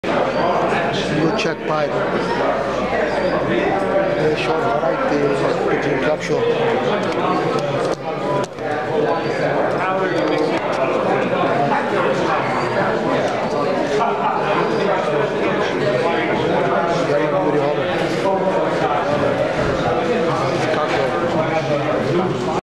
German Beauty Homers blue check pied
Blue check pied cock bird German Beauty Homers at Bayshore Variety pigeon club Show